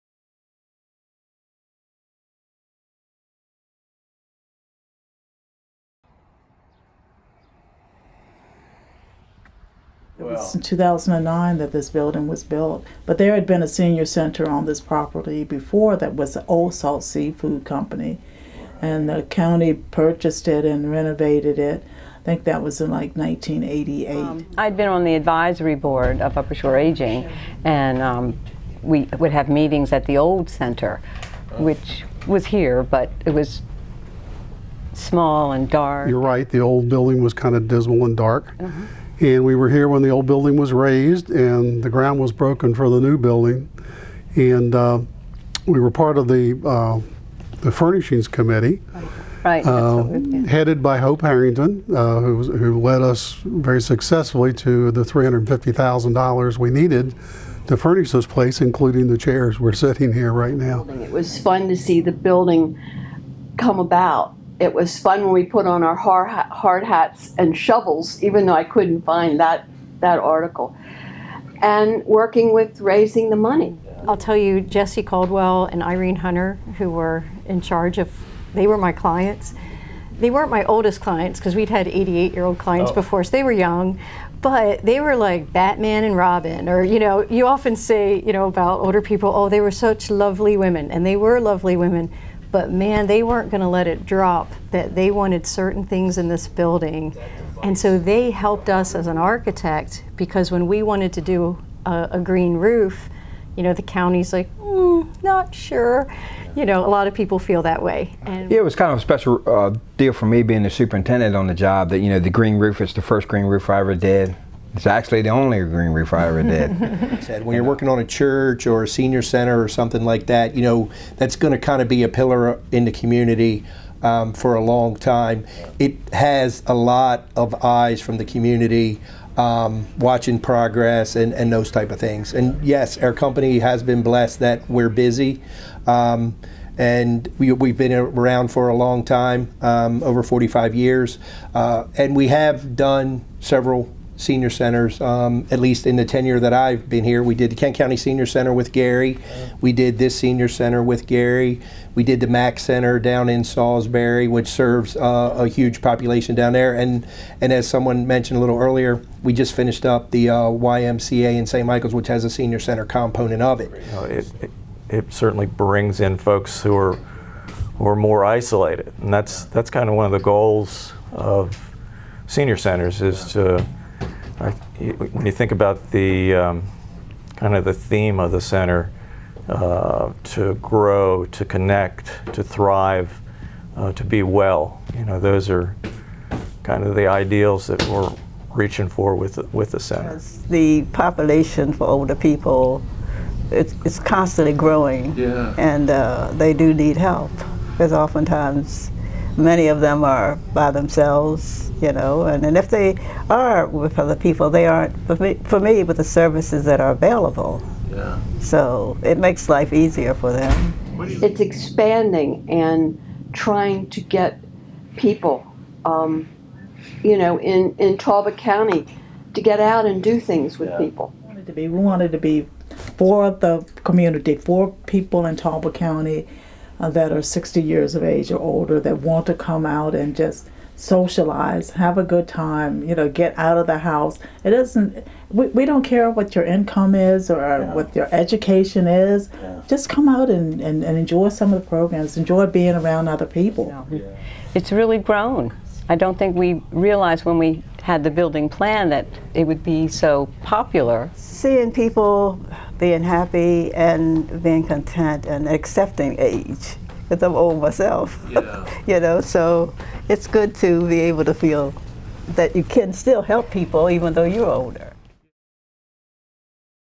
After the First Decade: Upper Shore Aging's ​Brookletts Place Celebrates Ten Years
As the Spy celebrates its tenth year of operation, it dawned on us that there were quite a number of enterprises on the Mid-Shore that started at the same time as the Spy.

Ranging from bakeries to nonprofit businesses and buildings, dozens of small businesses opened their doors amid a significant economic recession, relying on instinct and confidence that their services would be sought after, no matter the current business climate.With that in mind, the Spy has decided to interview many of these brave entrepreneurs and leaders over the next year for them to reflect on their experiences.

We continue with our chat with the staff, designers, and and volunteer leaders who made Brookletts Place - Upper Shore Aging's senior center for Talbot County a reality ten years ago.